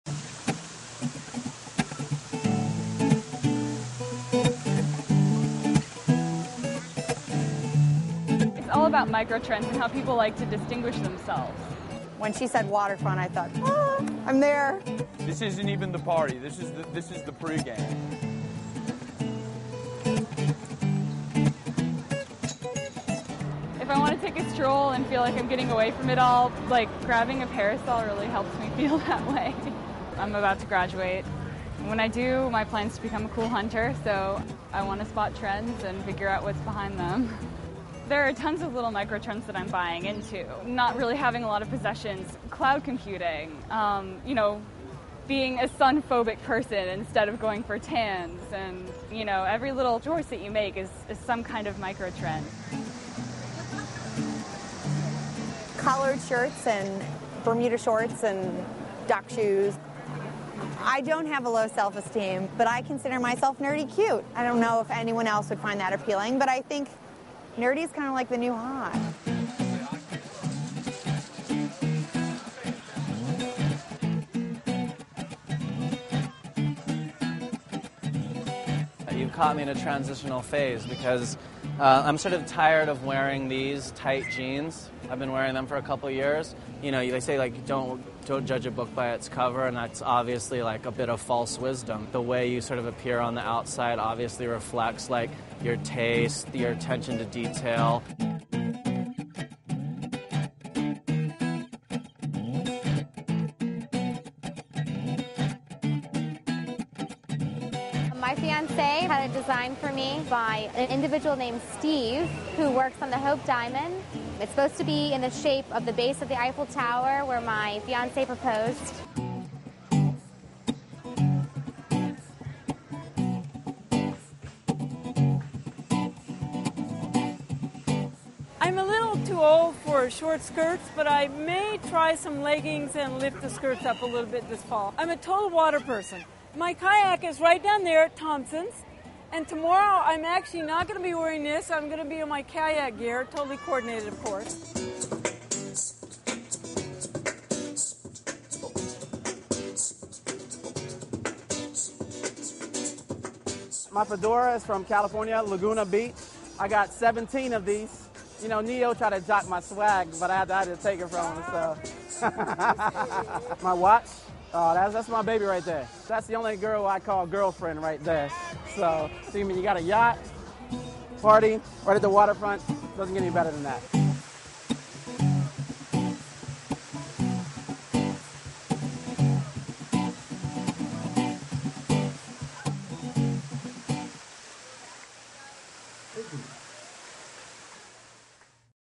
Scene In: Georgetown Waterfront
The sixth episode in a video series exploring how D.C. denizens show their sense of style. The style-conscious flock to Georgetown's waterfront to catch the lingering rays of sunny weather. Video by Alexandra Garcia / The Washington Post. for the full series.